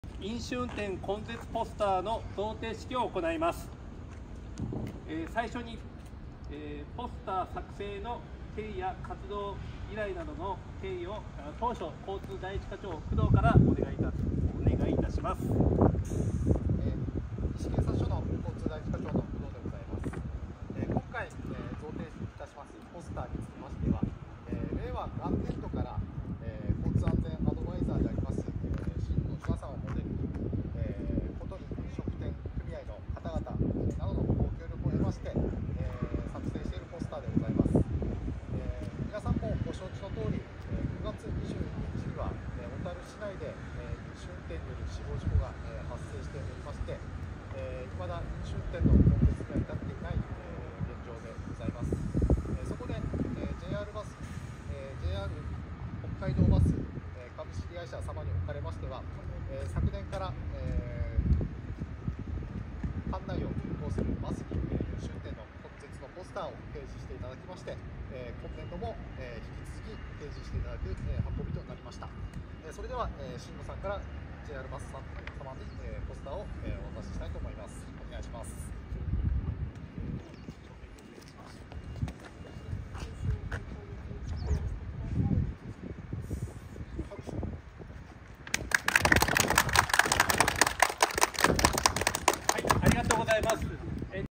令和6年 ジェイアール北海道バス 飲酒運転根絶ポスター贈呈式
2024年9月30日14：00実施

ジェイアール北海道バス　本社敷地内